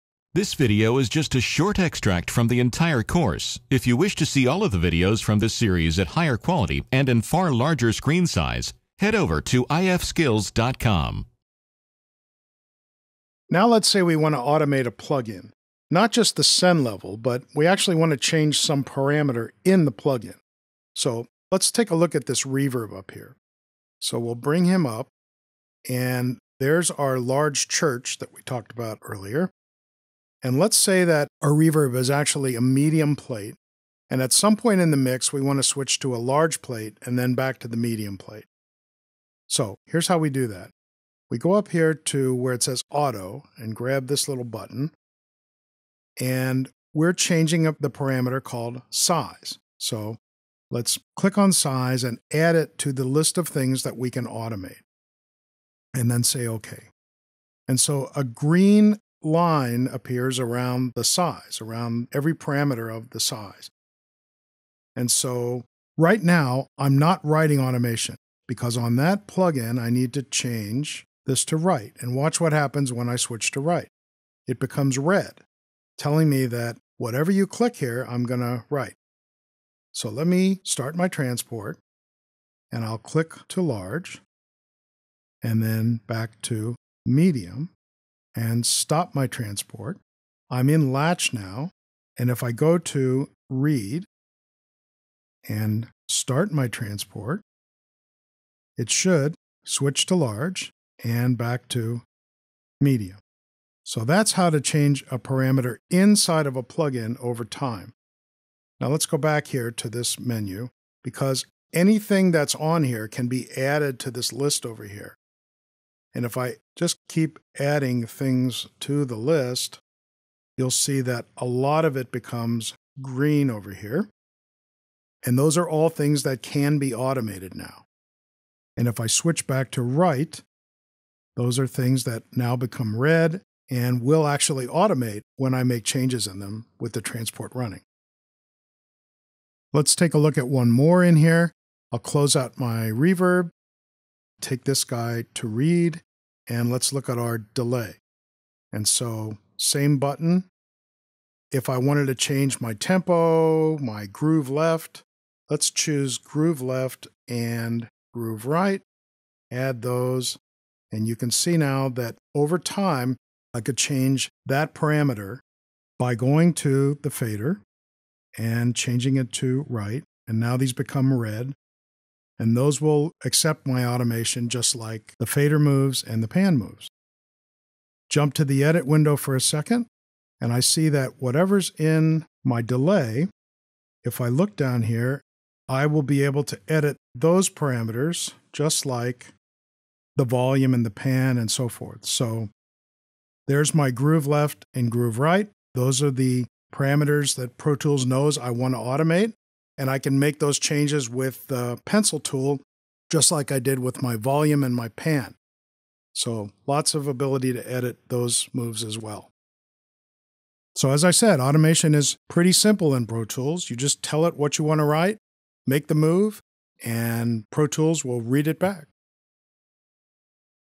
Pro Tools 9 Tutorial - Automating Plugin Parameters
Want all of our free Pro Tools videos?

Pro Tools 9 Tutorial Video on Automating Plugin Parameters. This video is taken from our Pro Tools 9 Training Course YouTube